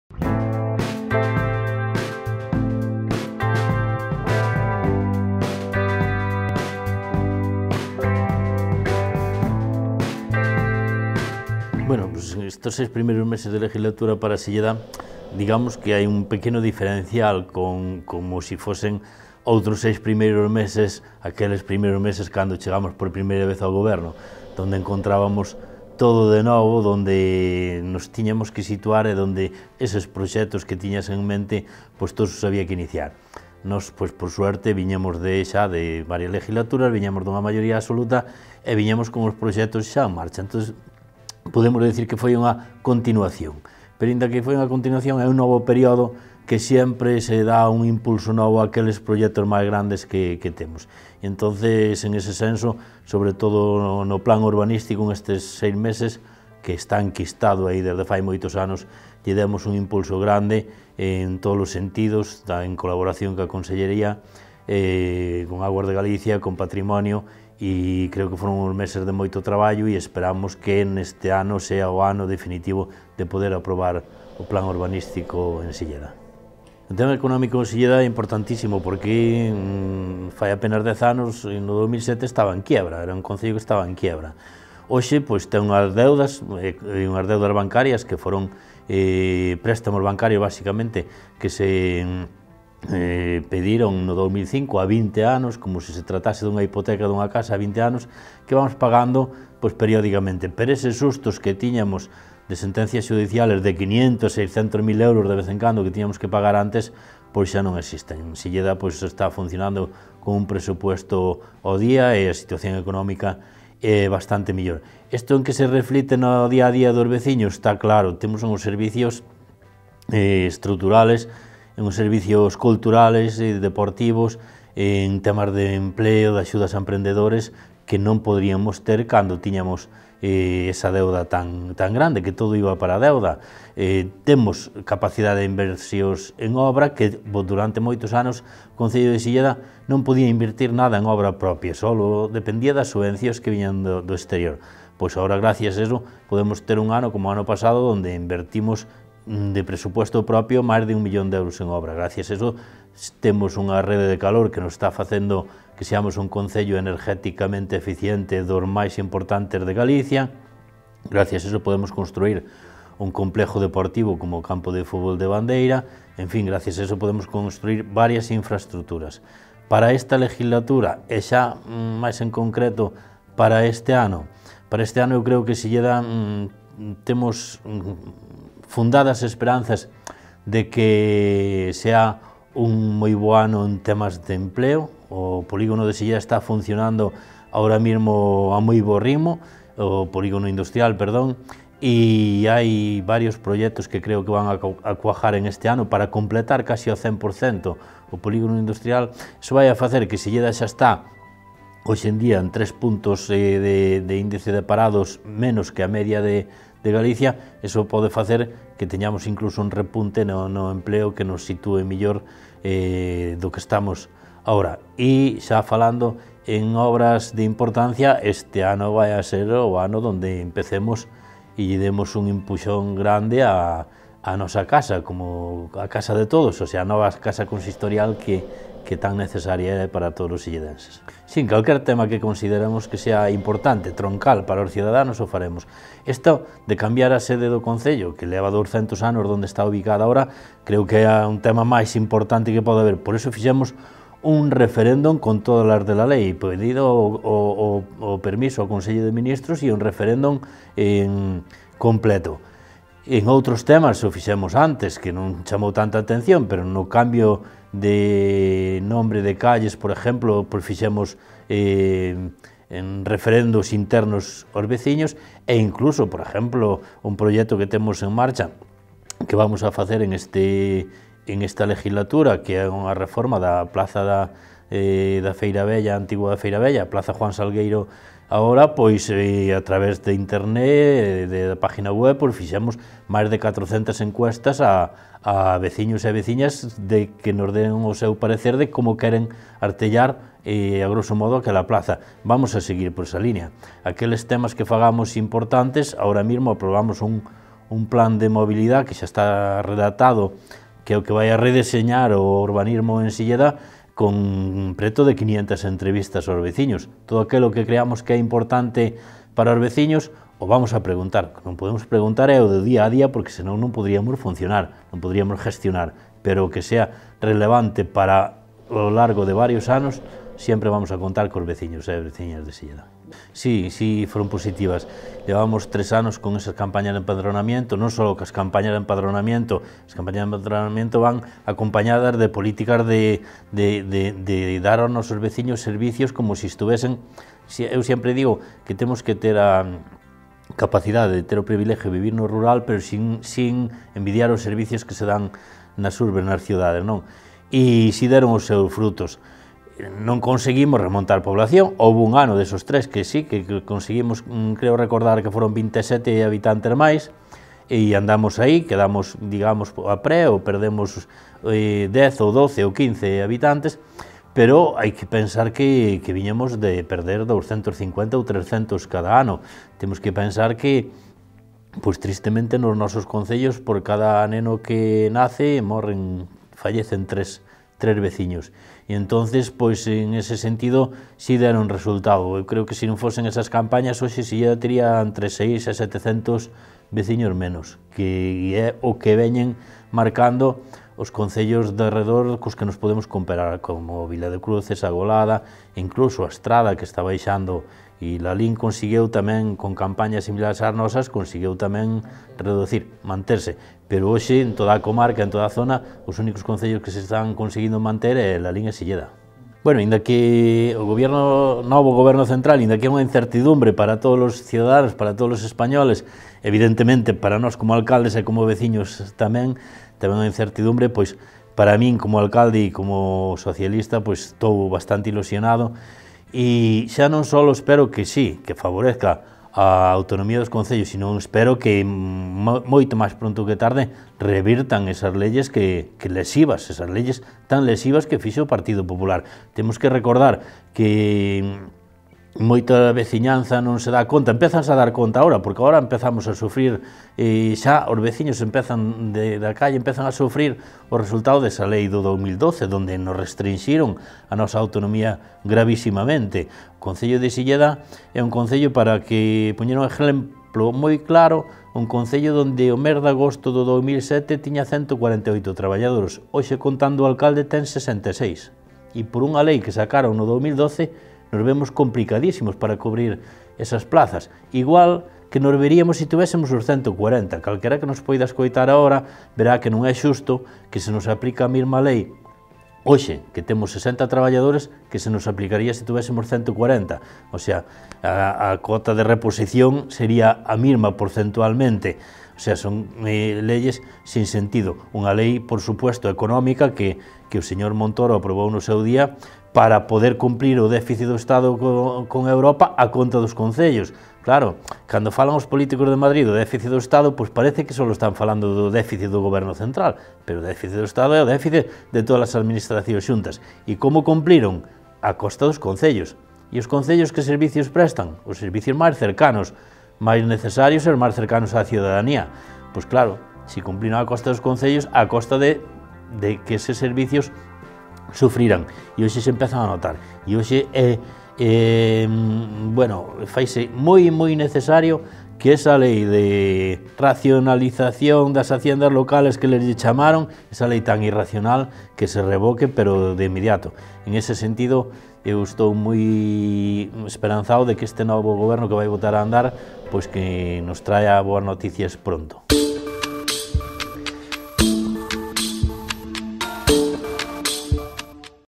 Visita do alcalde de Silleda a El Correo Gallego
Manuel Cuiña, alcalde de Silleda, visita as instalacións de El Correo Gallego